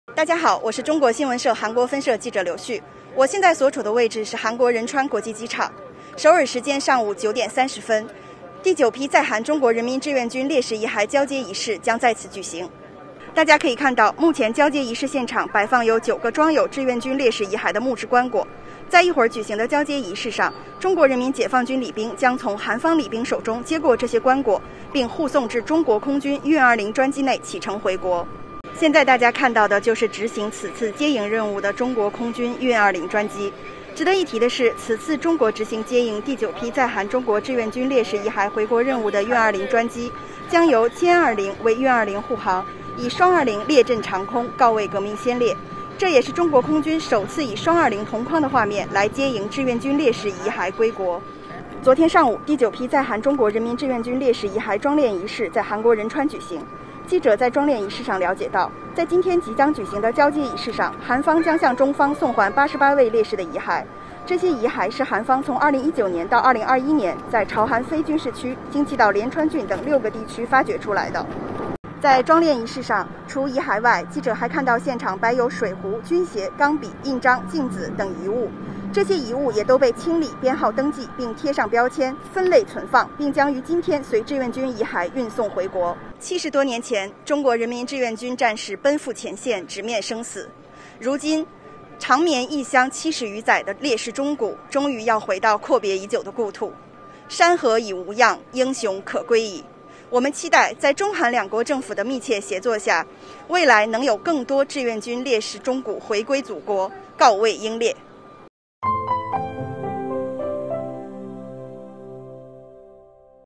88位志愿军烈士遗骸即将回国！“双20”列阵长空告慰英烈
中韩双方9月16日将在韩国仁川国际机场举行遗骸交接仪式。同日，中方将在辽宁沈阳桃仙国际机场举行迎回仪式，并于17日在沈阳抗美援朝烈士陵园举行安葬仪式。